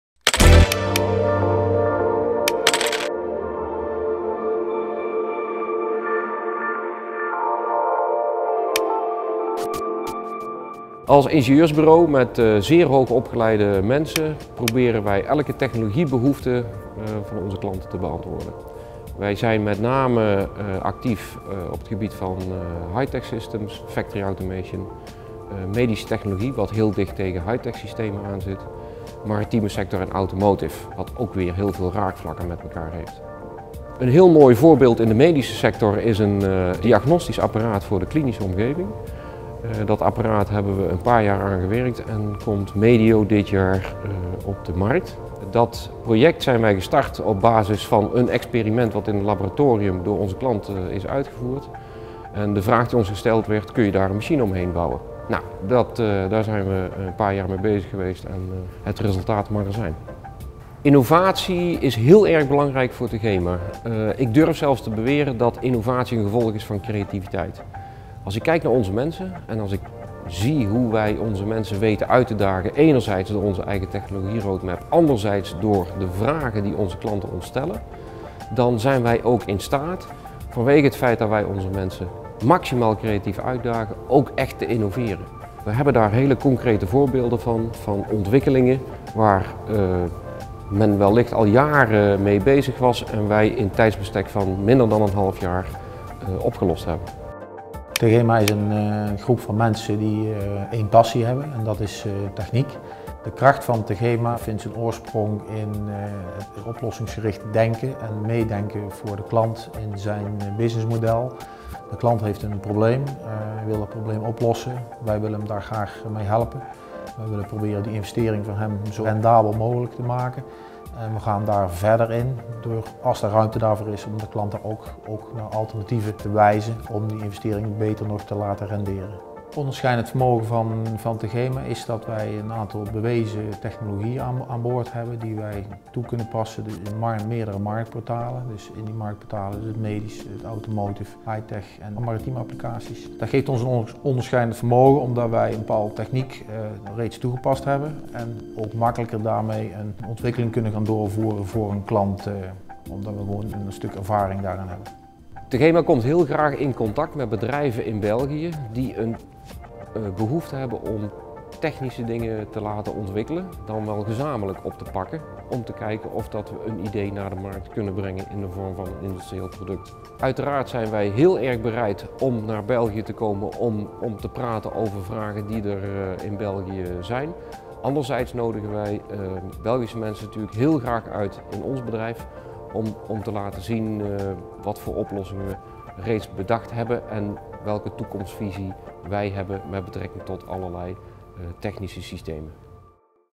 Tegema
Tegema is een mechatronische projectenorganisatie op het gebied van product- en systeemontwikkeling, productieautomatisering en bedrijfsmechanisatie.
Tegema ontwikkelt, innoveert en realiseert oplossingen voor haar klanten in de brede markt van High-Tech Systems, Speciaal Machines en Automotive Systems.

Tegema heeft een sterke trackrecord in product- en systeemontwikkeling en Engineering Services. Zowel haalbaarheidsonderzoeken, concept ontwikkeling, value-engineering als de opzet van tooling en testmiddelen zijn bij de Tegema Group in goede handen.
Al ruim 30 jaar is de Tegema Group uw partner in het ontwikkelen en realiseren van totaaloplossingen.